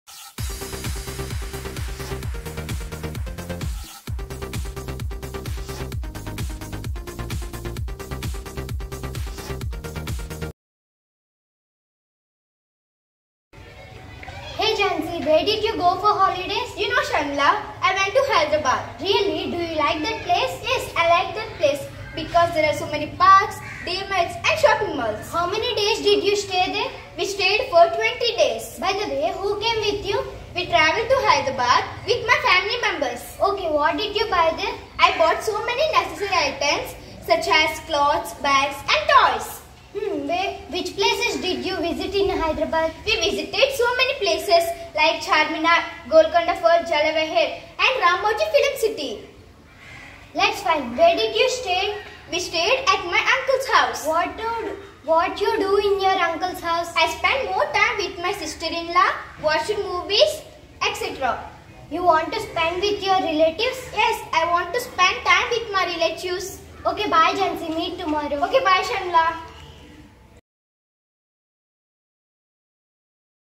Spoken English - A Trip to Hyderabad
It's a conversation between two students about a visit to Hyderabad.